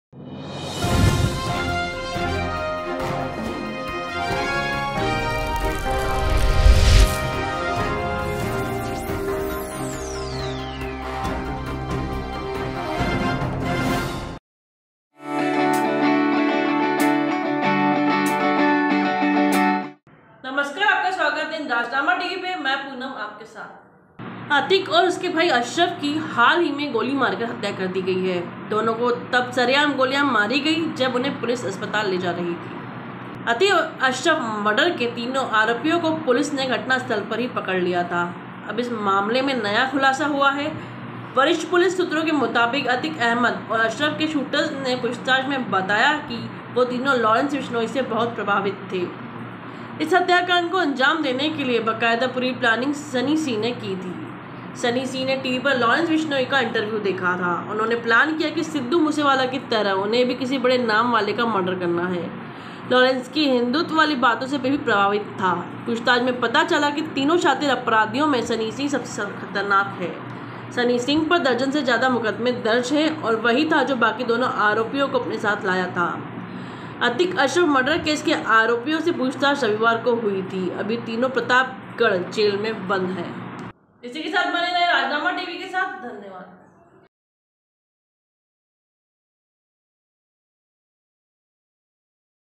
anchorpoonam अतीक अशरफ - मर्डर केस के शूटर्स लॉरेंस विश्नोई से थे प्रभावित
About this video: अतीक अशरफ - मर्डर केस के शूटर्स लॉरेंस विश्नोई से थे प्रभावित